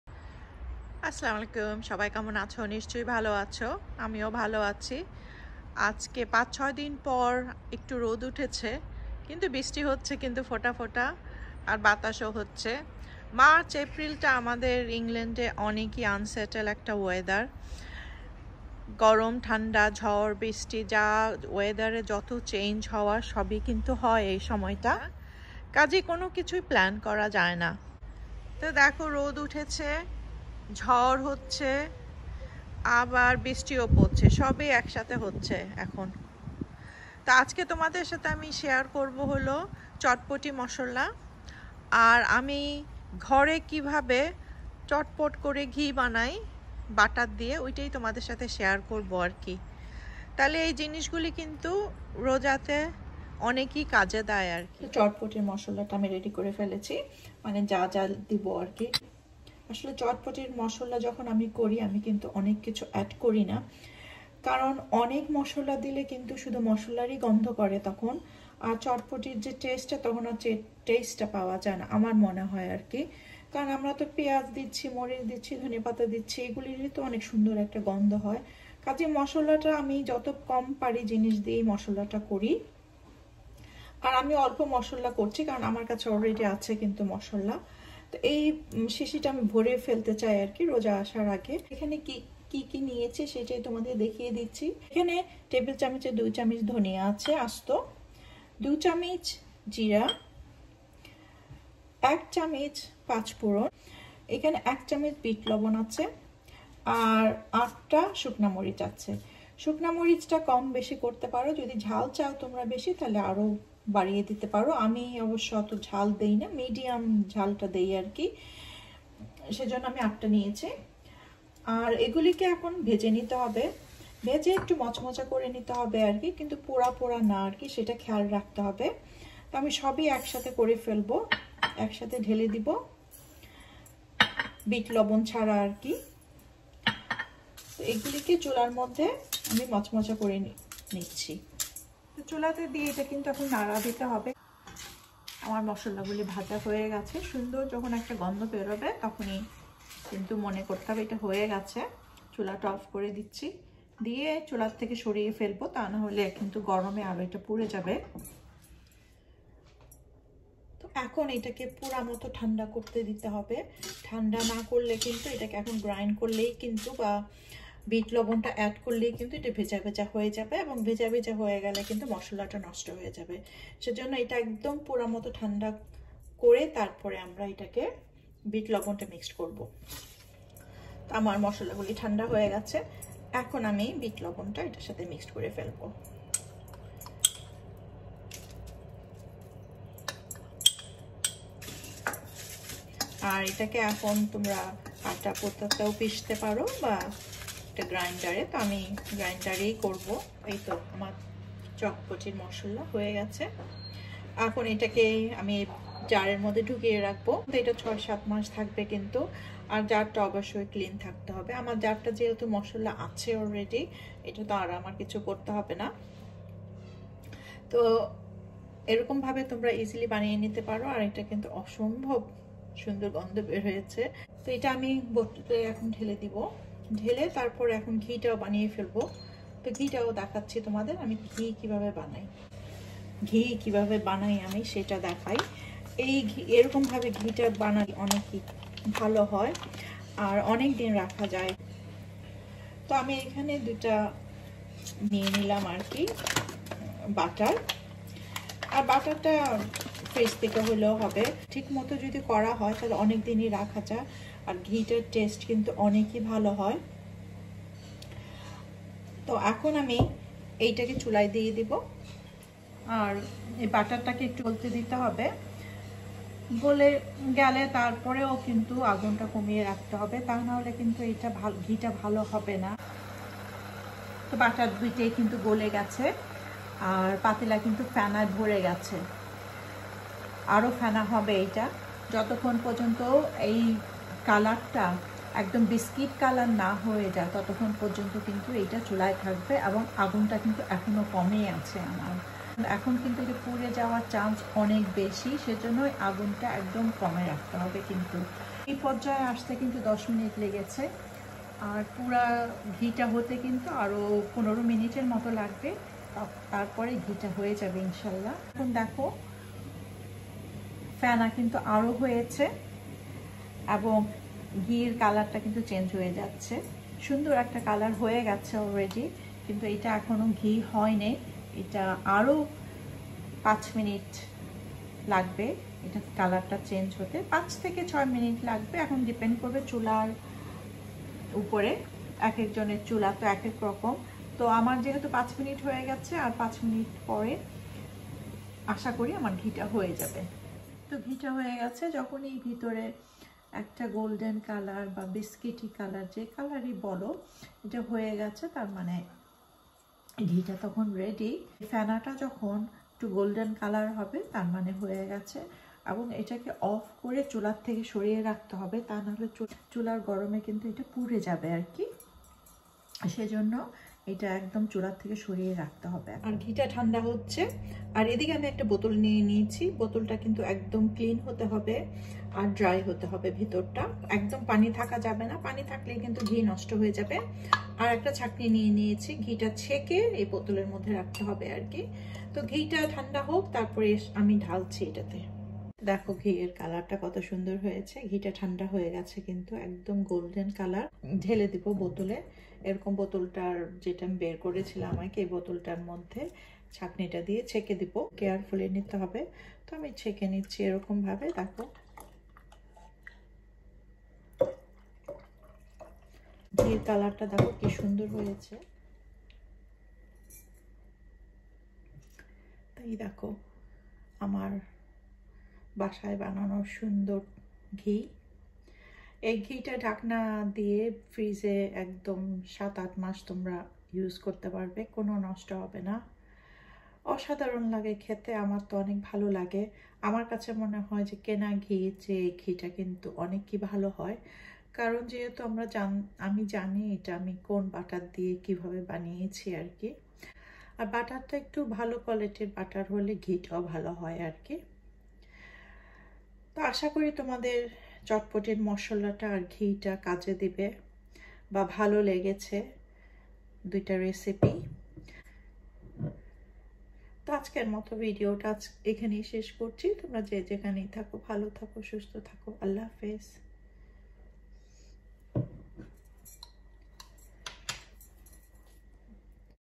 How to make Ghee from Butter and Chotpoti Masala Recipe || Bangla
Home made chotpoti Masala ana ghee recipe from Butter//. Bengali recipe